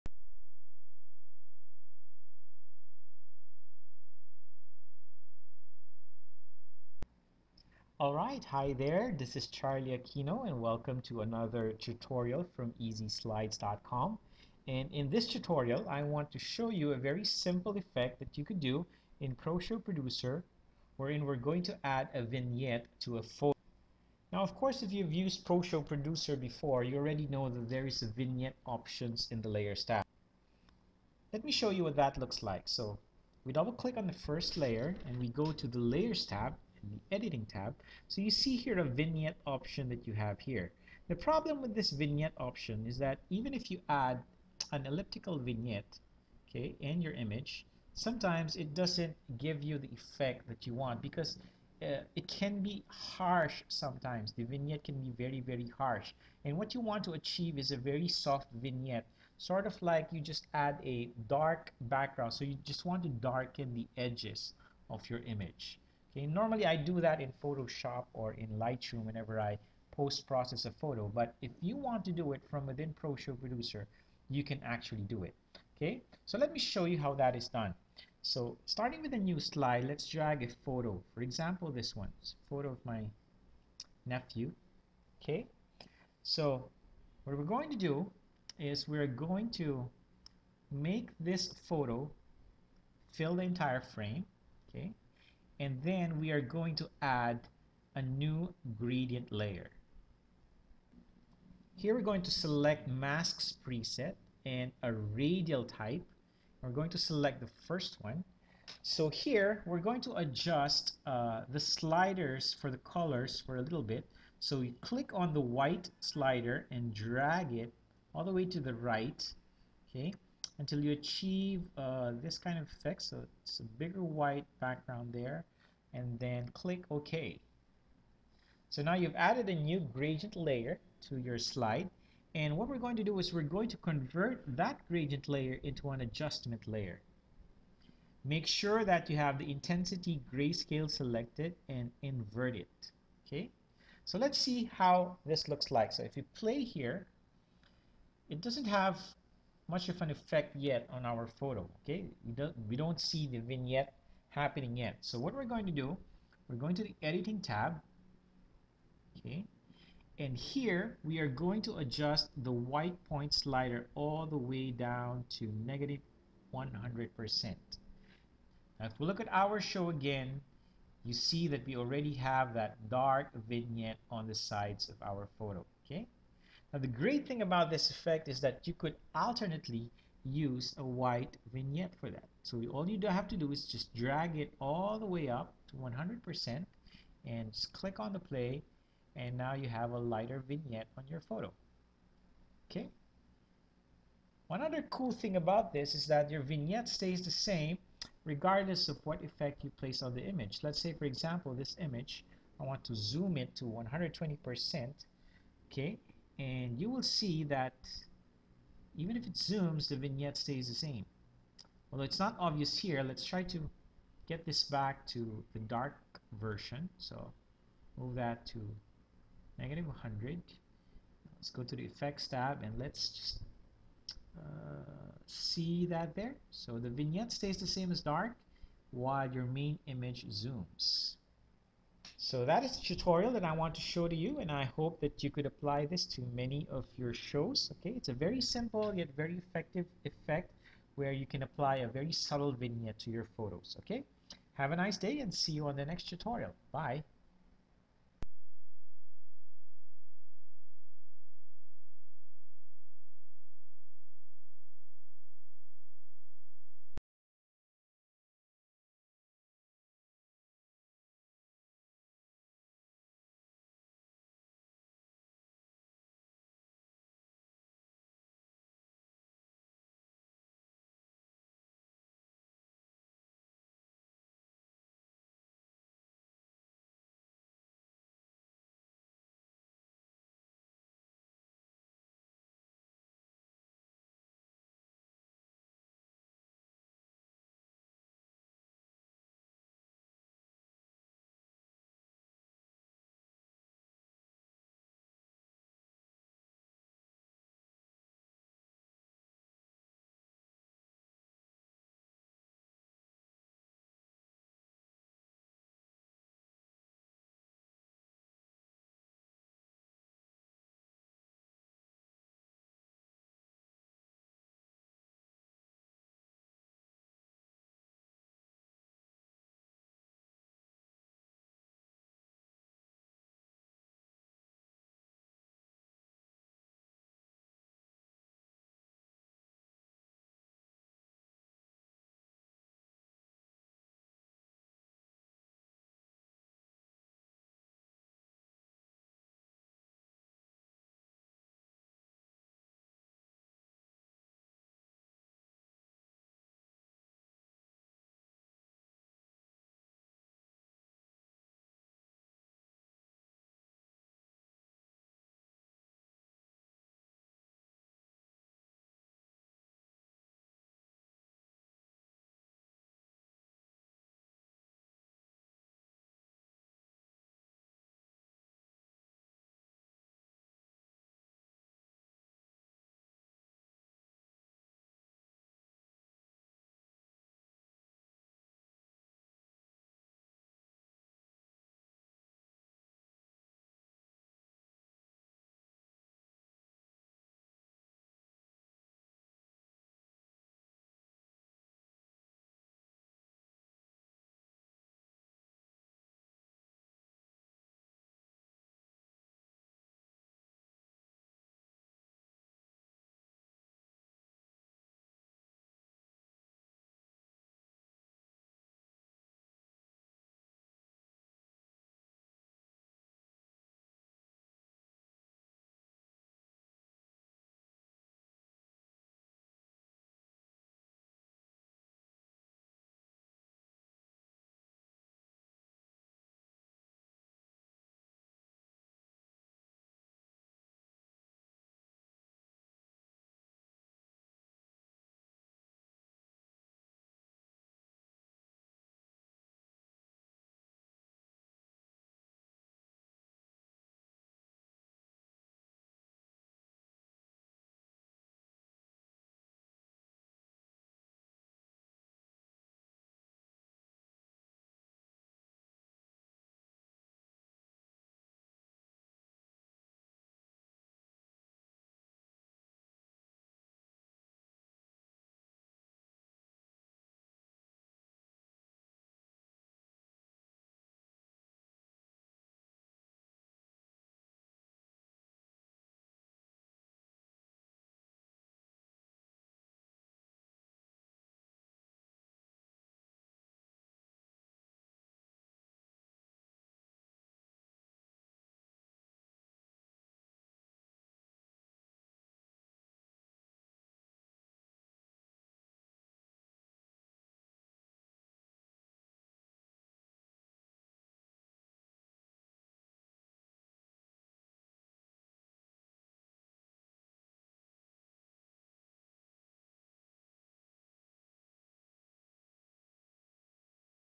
Proshow Tutorial - Subtle Vignette Effect
- Proshow Producer tutorial to add a subtle light or dark vignette on a photo